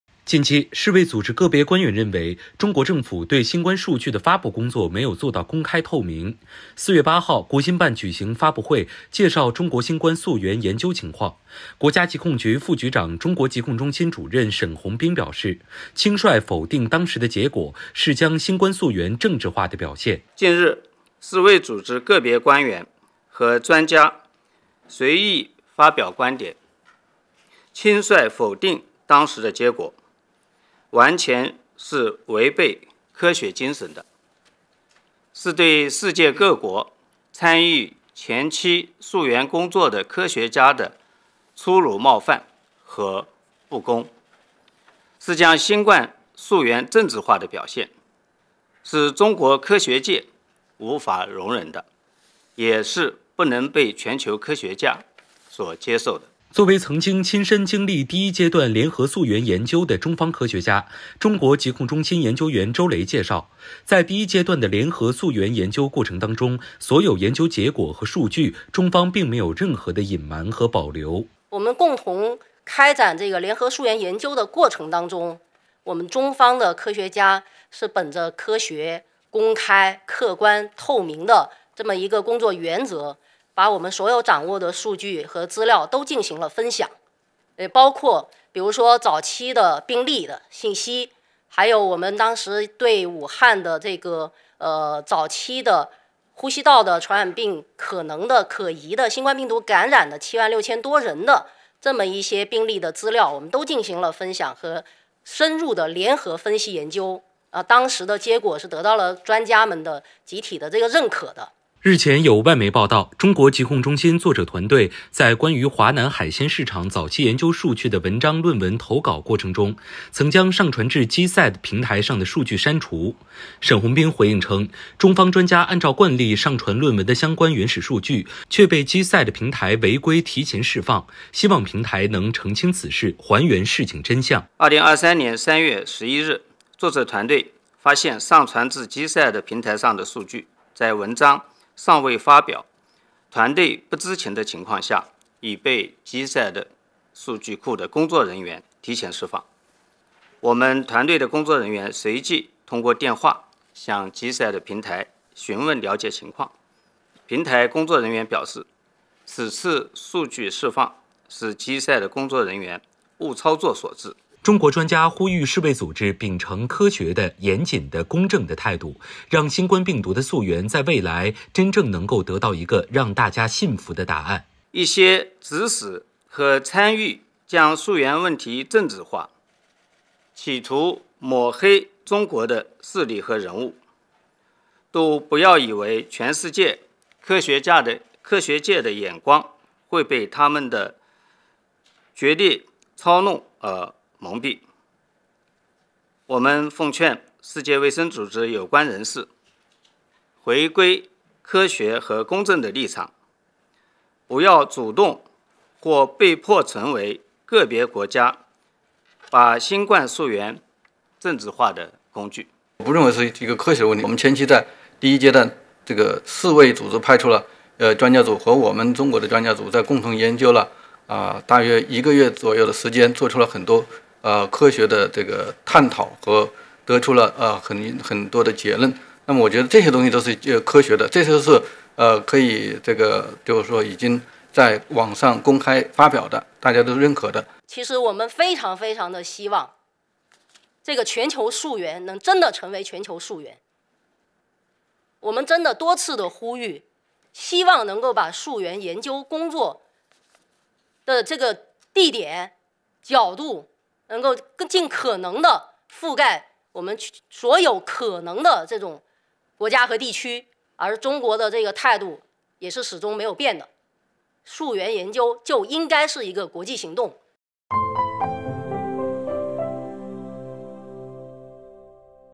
世卫组织个别官员和专家否定溯源结果 中方回应
近期，世卫组织个别官员认为，中国政府对新冠数据的发布工作没有做到公开透明。4月8日，国新办举行发布会，介绍中国新冠溯源研究情况。国家疾控局副局长、中国疾控中心主任沈洪兵表示，轻率否定当时的结果，是将新冠溯源政治化的表现。